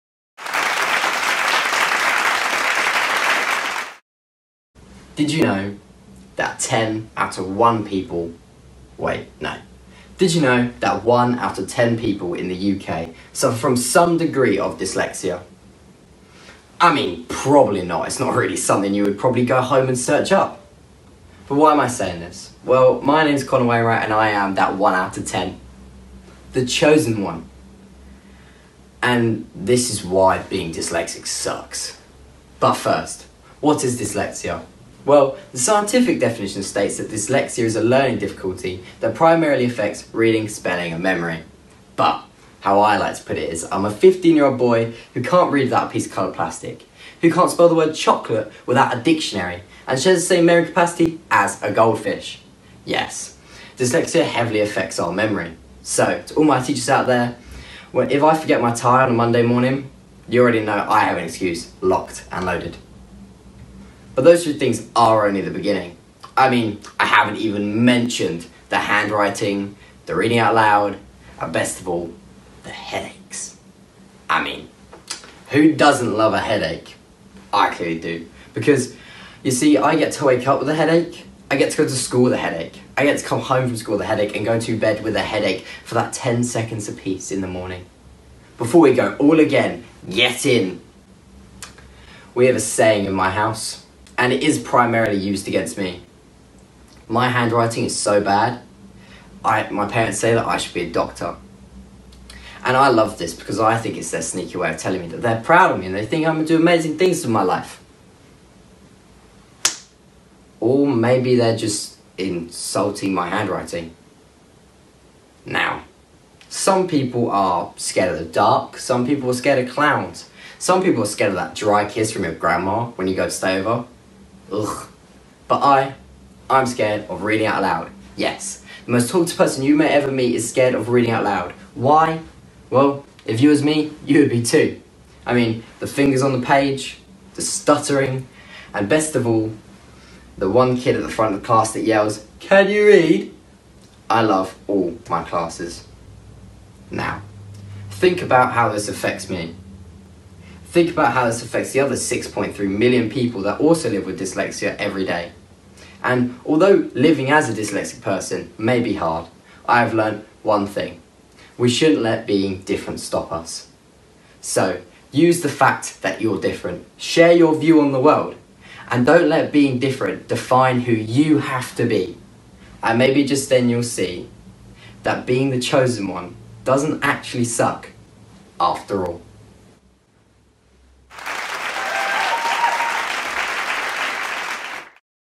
Conall Wainwright, The Chosen One, JPSpeakOut
Conall Wainwright of Chelmer Valley High School became the winner of this “Speak Out" Challenge! Digital Final with a speech titled 'The Chosen One'.
This final took place via Zoom Webinar on Thursday 15th July 2021.